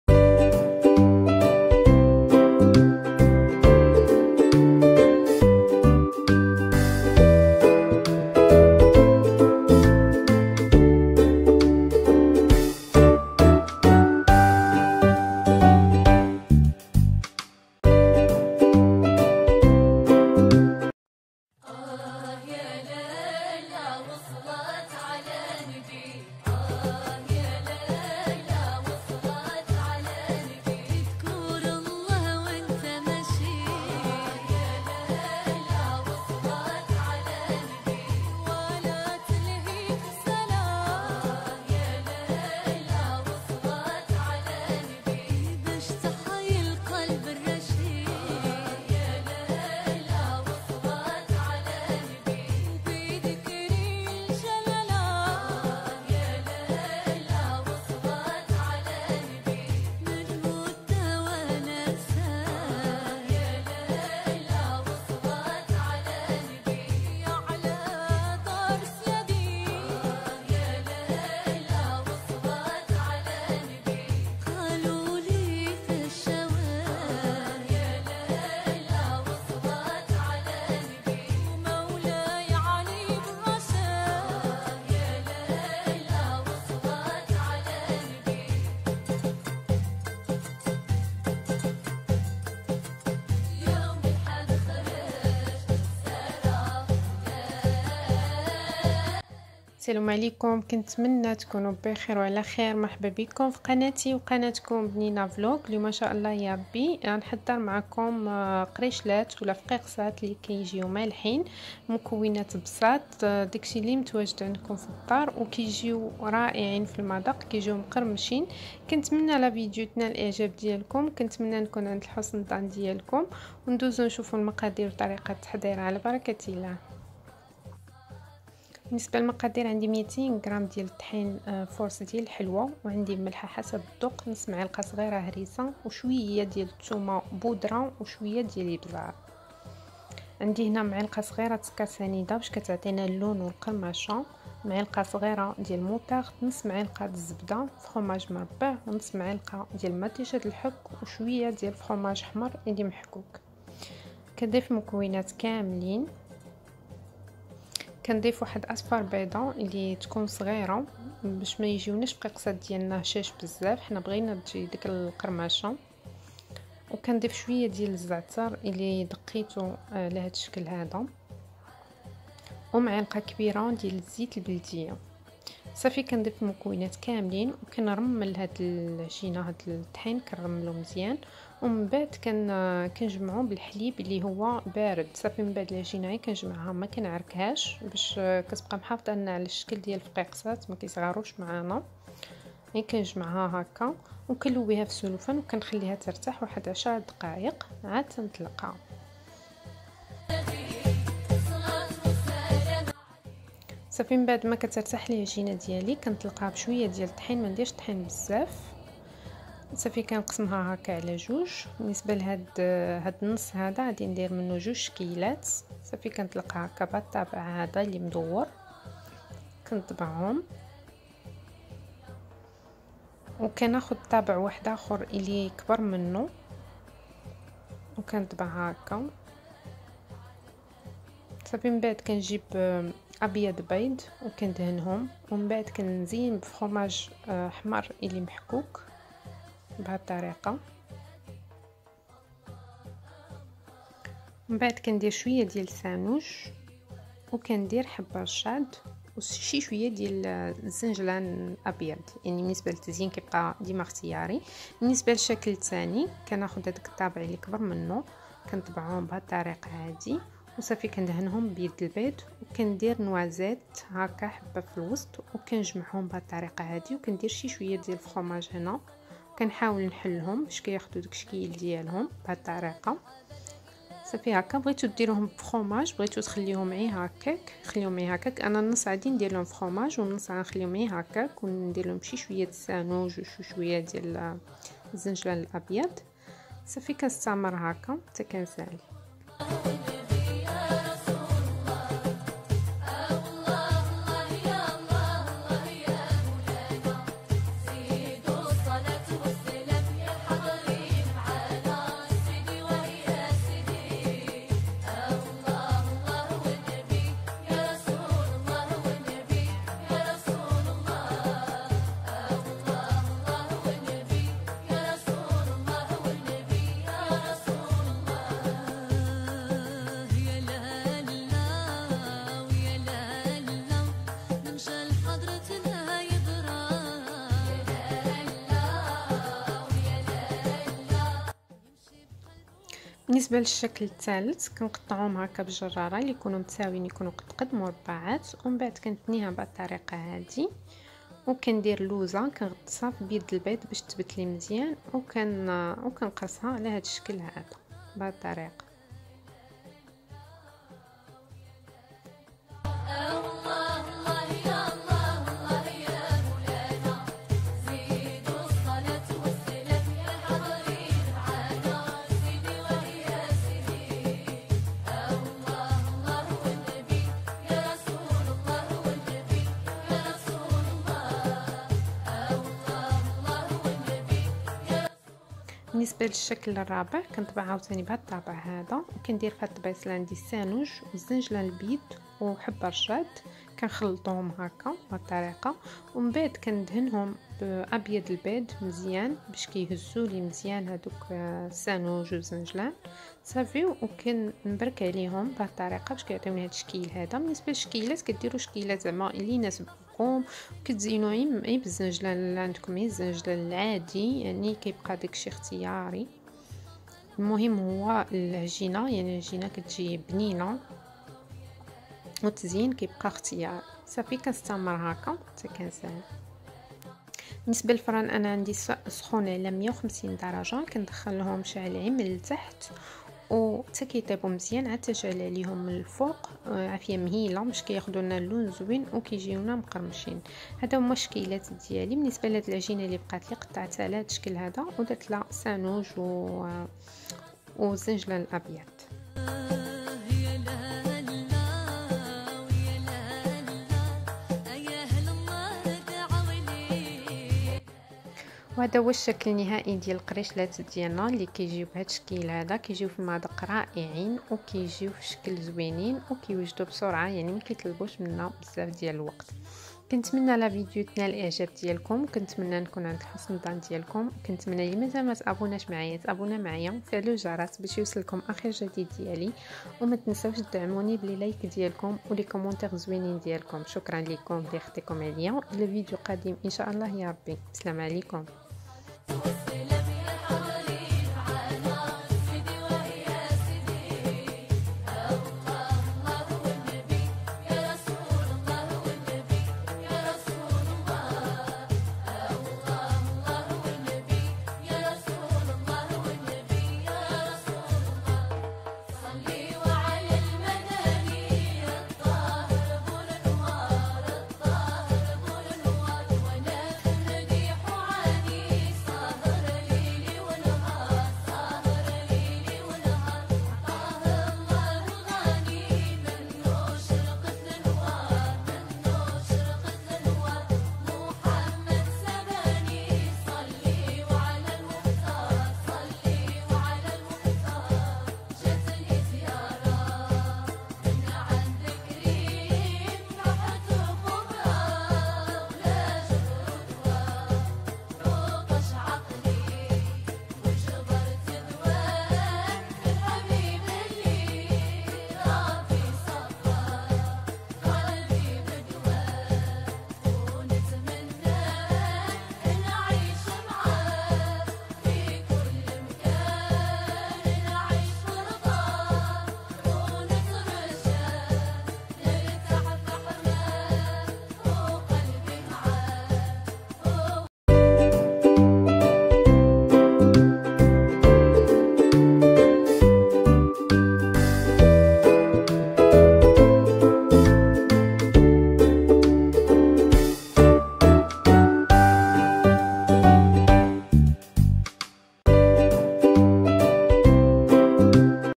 قريشلات/فقيقصات مالحين حضريهم بسرعة
قريشلات
200ج طحين

ملح  الحار ثوم  بودرة  ابزار
م ص سنيدة
م ص خردل
نصف م ص  زبدة مطيشة حك
1 فرماج  كيري
فرماج احمر

اصفر بيض
زعتر
م ك زيت بلدية
نصف كأس  حليب